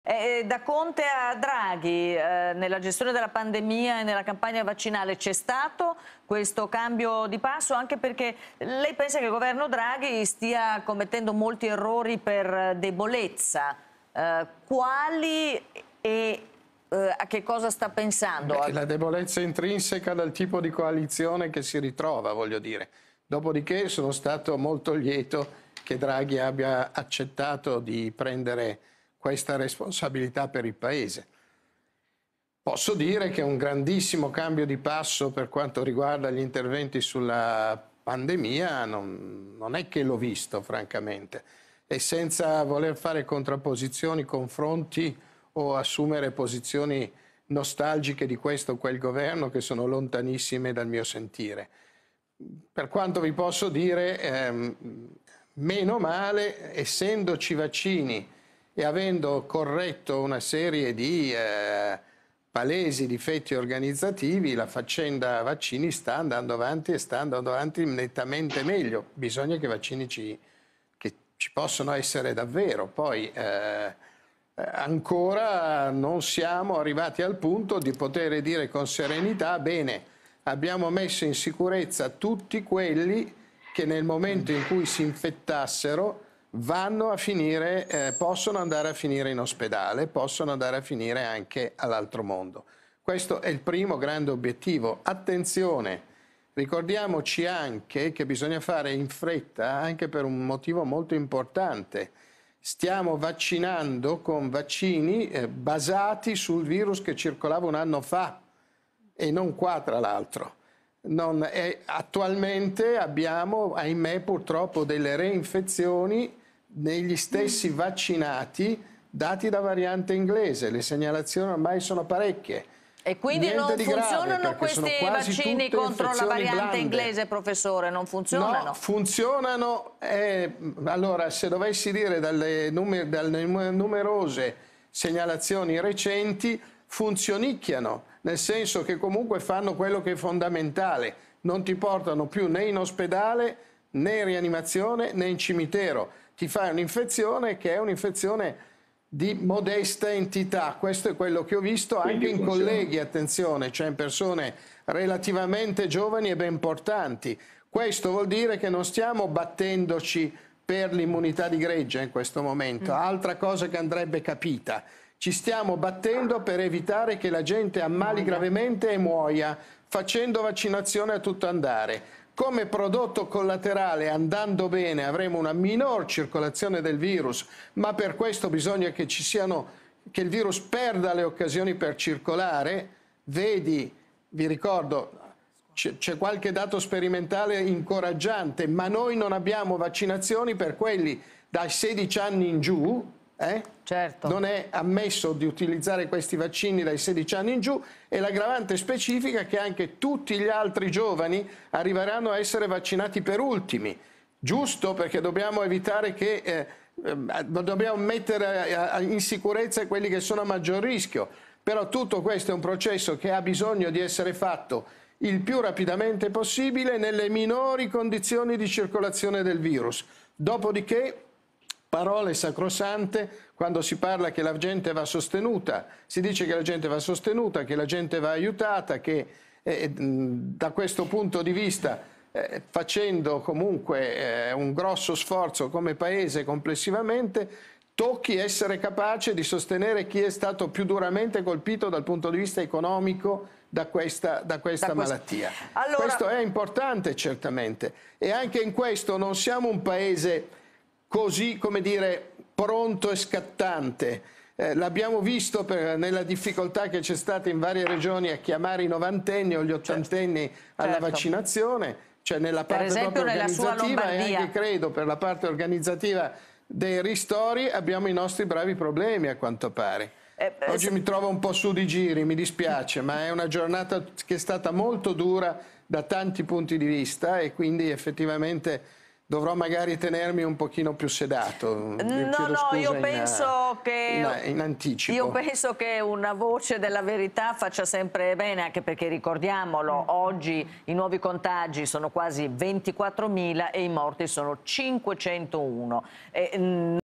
Vaccino anti-covid, la rivelazione del Prof. Galli: "Contro la variante inglese ...
Vaccino anti-covid, la rivelazione del Prof. Galli: "Attualmente abbiamo delle reinfezioni negli stessi vaccinati dati da varianti inglesi, le segnalazioni sono parecchie. Il  vaccino contro la variante inglese 'funzionicchia'"